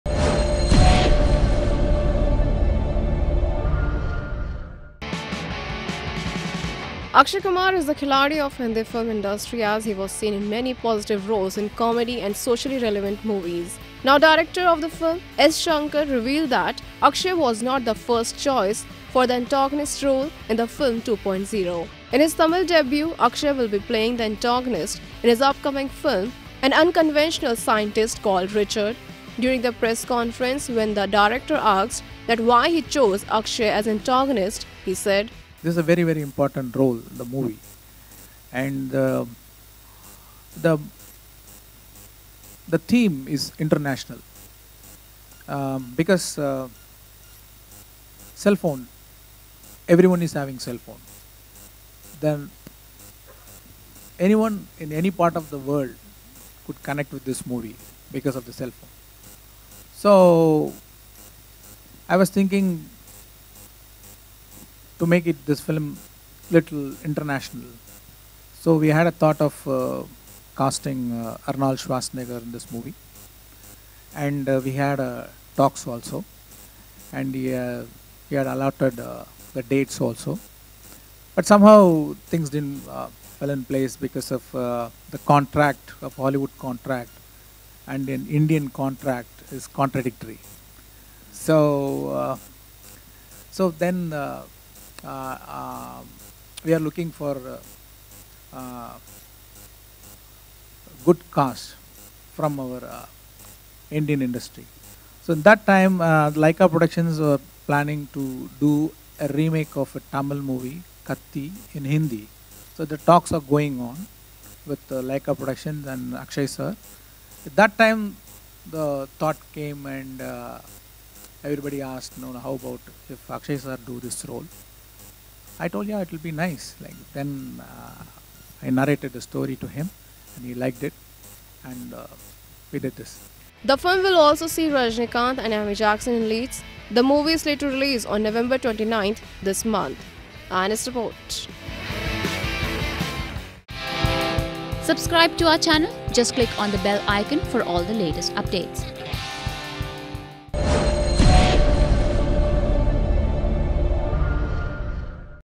Not Akshay, Arnold was Director’s first choice for ‘2.0’
Akshay Kumar is the Khiladi of hindi film industry as he was seen in many positive roles in comedy and socially relevant movies. Now, Director of the film S. Shankar revealed that Akshay was not the first chocie for the Antagonist role in the film ‘2.0’.  The film will also see Rajinikanth and amy jackson in leads.